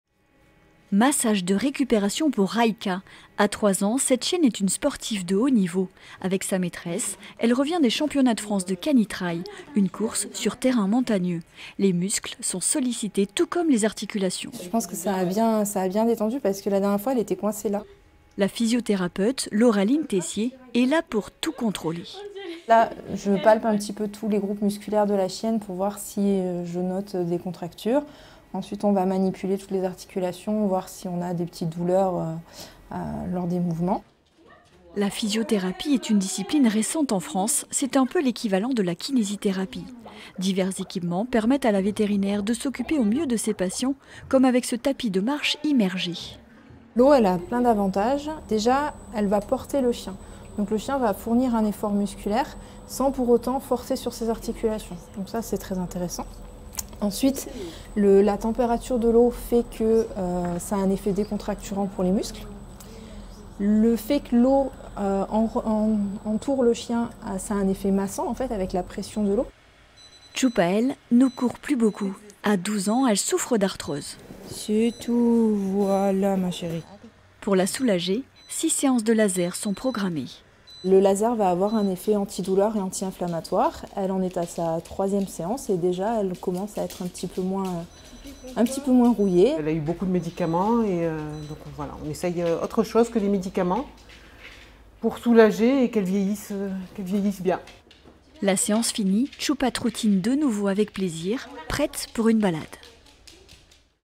Une physiothérapeute pour les animaux dans le Tarn
Soulager les douleurs d'arthrose, améliorer la mobilité, aider à la récupération musculaire, chiens, chats et autres animaux de compagnie ont désormais leur cabinet de physiothérapie à Labruguière. Le premier du Tarn. Une jeune vétérinaire s'est installée en octobre pour proposer ces soins équivalents à la kinésithérapie.